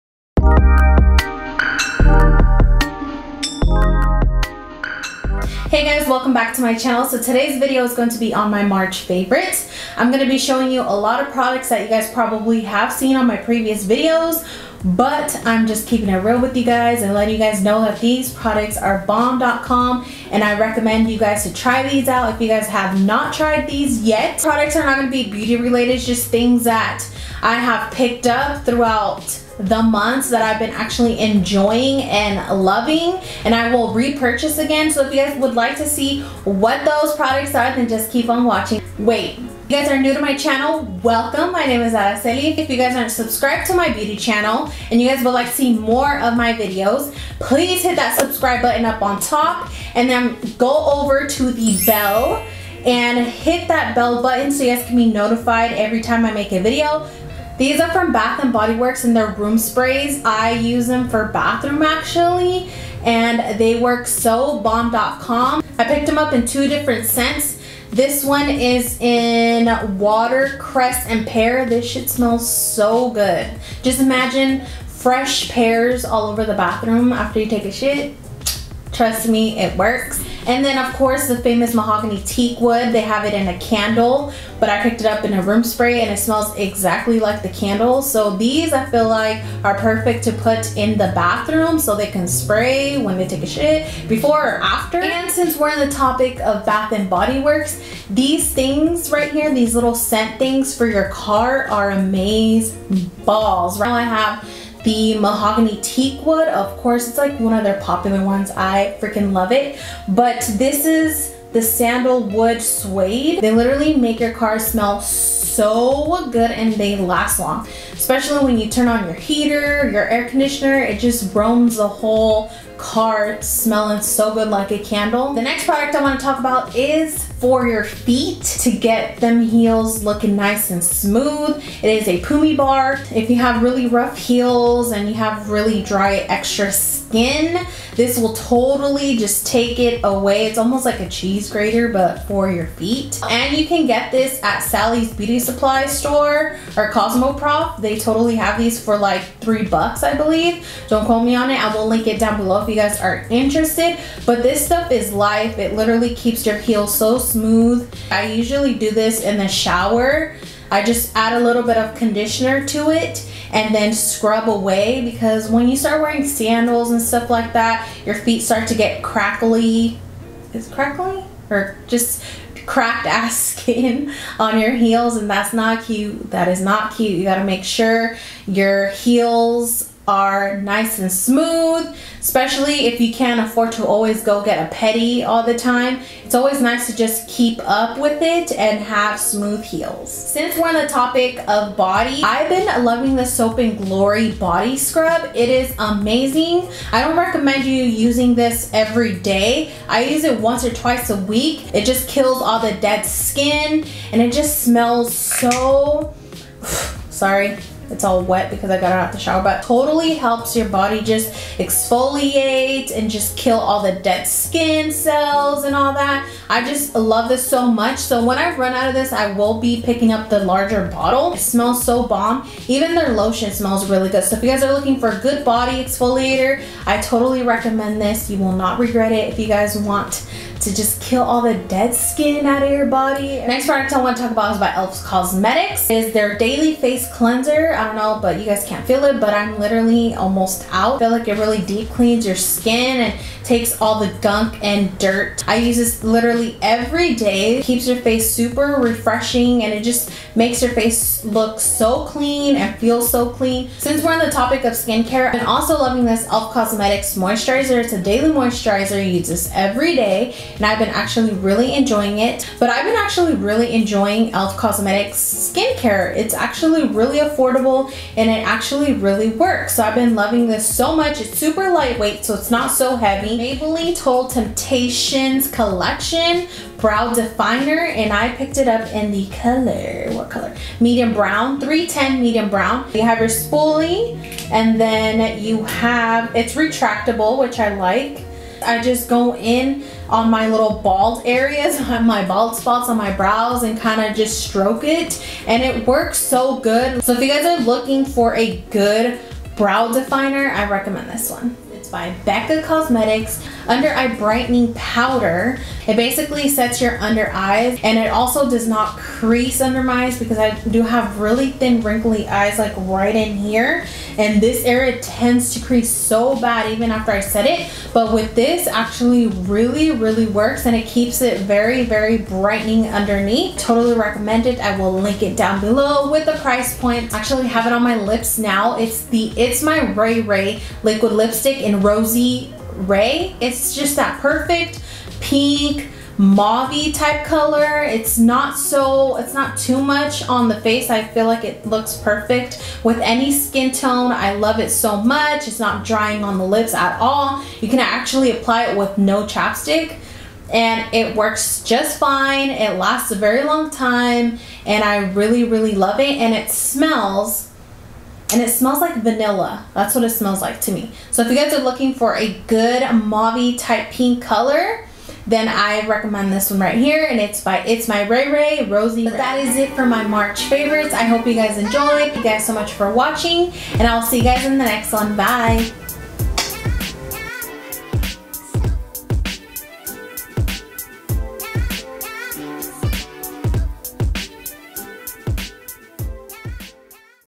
March Beauty Favorites 2018 | Aracelibeautyy89
HEY SUBBIE SQUAD I AM HERE TO SHARE WITH YOU MY BEAUTY FAVORITES FOR THE MONTH OF MARCH! I HOPE YOU GUYS ENJOY

LET ME KNOW WHAT YOUR FAVORITES ARE FOR THE MONTH OF MARCH?

PURCHASE YOUR PUMI BAR HERE FOR YOUR FEET $1.29

VERO: Aracelibeautyy89